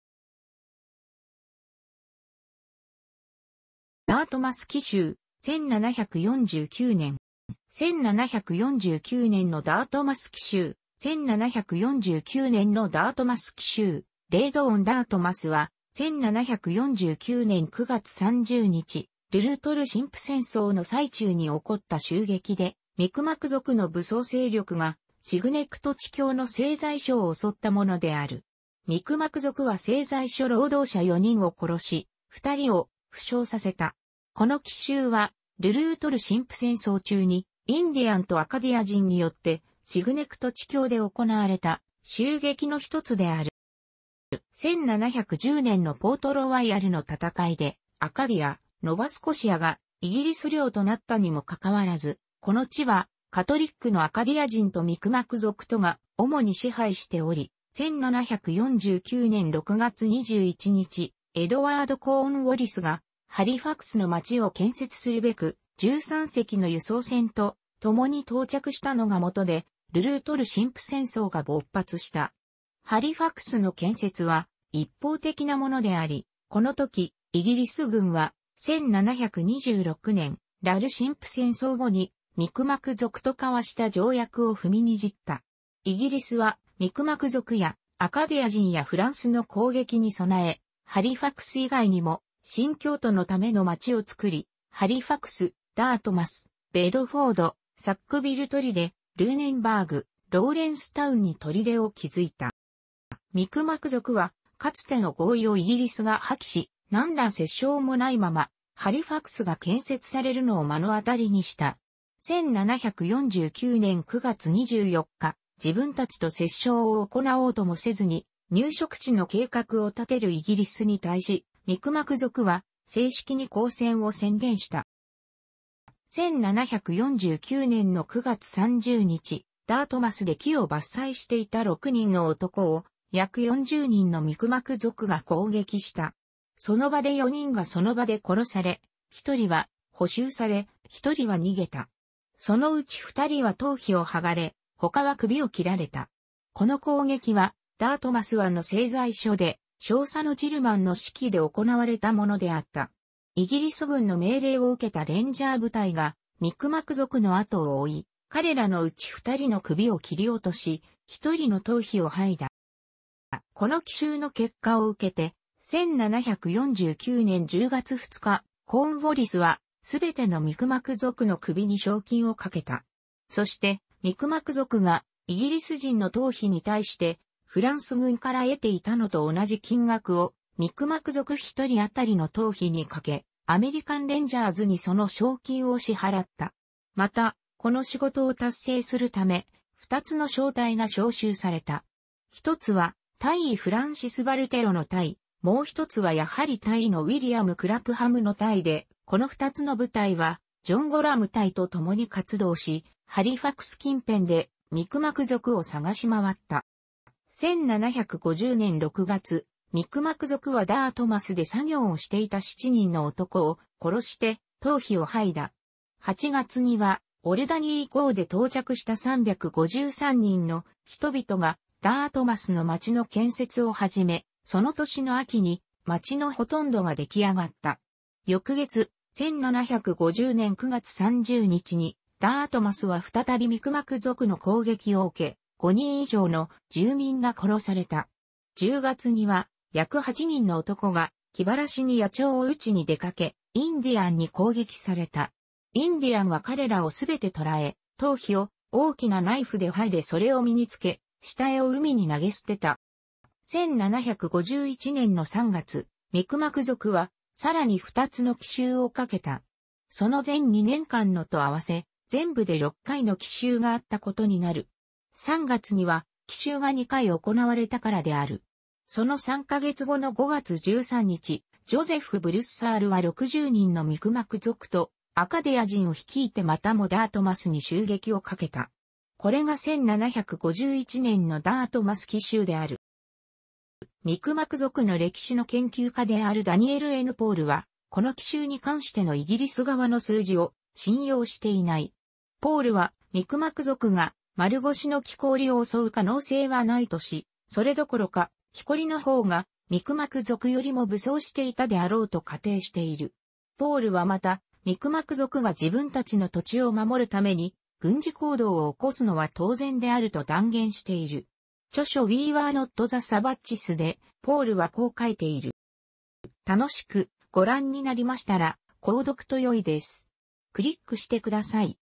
ダートマス奇襲 (1749年)
1749年のダートマス奇襲（1749ねんのダートマスきしゅう、Raid on Dartmouth）は、1749年9月30日、ル・ルートル神父戦争の最中に起こった襲撃で、ミクマク族の武装勢力が、シグネクト地峡の製材所を襲ったものである。ミクマク族は製材所労働者4人を殺し、2人を負傷させた。この奇襲は、ル・ルートル神父戦争中に、インディアンとアカディア人によって、シグネクト地峡で行われた襲撃の一つである。

1710年のポートロワイヤルの戦いで、アカディア（ノバスコシア）がイギリス領となったにもかかわらず、この地はカトリックのアカディア人とミクマク族とが主に支配しており、1749年6月21日、エドワード・コーンウォリスが、ハリファクスの町を建設するべく、13隻の輸送船と共に到着したのがもとでル・ルートル神父戦争が勃発した 。ハリファクスの建設は一方的なものであり、この時イギリス軍は、1726年、ラル神父戦争後に、ミクマク族とかわした条約を踏みにじった 。イギリスは、ミクマク族や、アカディア人やフランスの攻撃に供え、ハリファクス以外にも新教徒のための町を作り、ハリファクス、ダートマス、ベドフォード（サックヴィル砦）、ルーネンバーグ、ローレンスタウンに砦を築いた。

ミクマク族は、かつての合意をイギリスが破棄し、何ら折衝もないまま、ハリファクスが建設されるのを目の当たりにした。1749年9月24日、自分たちと折衝を行おうともせずに、入植地の計画を立てるイギリスに対し、ミクマク族は正式に交戦を宣言した。

1749年の9月30日、ダートマスで木を伐採していた6人の男を、約40人のミクマク族が攻撃した。その場で4人がその場で殺され、1人は捕囚され、1人は逃げた。そのうち2人は頭皮を剥がれ、他は首を切られた。この攻撃は、ダートマス湾の製材所で、少佐のジルマンの指揮で行われたものであった。イギリス軍の命令を受けたレンジャー部隊がミクマク族のあとを追い、彼らのうち2人の首を切り落とし、1人の頭皮を剥いだ。

この奇襲の結果を受けて、1749年10月2日、コーンウォリスはすべてのミクマク族の首に賞金を懸けた。そして、ミクマク族が、イギリス人の頭皮に対して、フランス軍から得ていたのと同じ金額を、ミクマク族1人あたりの頭皮にかけ、アメリカンレンジャーズにその賞金を支払った。また、この仕事を達成するため、2つの小隊が召集された。1つは大尉フランシス・バルテロの隊、もう1つはやはり大尉のウィリアム・クラプハムの隊で、この2つの部隊は、ジョン・ゴラム隊と共に活動し、ハリファクス近辺でミクマク族を捜しまわった。

1750年6月、ミクマク族はダートマスで作業をしていた7人の男を殺して頭皮を剥いだ 。8月にはオルダニー号で到着した353人の人々が、ダートマスの町の建設を始め、その年の秋に町のほとんどが出来上がった。翌月、1750年9月30日にダートマスは再びミクマク族の攻撃を受け、5人以上の住民が殺された。10月には約8人の男が「気晴らしに野鳥を撃ちに出かけ、インディアンに攻撃された。インディアンは彼らをすべて捕らえ、頭皮を大きなナイフで剥いでそれを身に付け、死体を海に投げ捨てた…」

1751年の3月、ミクマク族はさらに2つの奇襲をかけた。その前2年間のとあわせ、全部で6回の奇襲があったことになる。3月には奇襲が2回行われたからである。 。その3ヶ月後の5月13日、ジョゼフ・ブルッサールは60人のミクマク族とアカディア人を率いてまたもダートマスに襲撃をかけた。これが1751年のダートマス奇襲である。

ミクマク族の歴史の研究家であるダニエル・N・ポールは、この奇襲に関してのイギリス側の数字を信用していない。ポールは、ミクマク族が丸腰の木こりを襲う可能性はないとし、それどころか、木こりの方がミクマク族よりも武装していたであろうと仮定している。ポールはまた、ミクマク族が自分たちの土地を守るために軍事行動を起こすのは当然であると断言している。著書"We Were Not the Savages"で、ポールはこう書いている。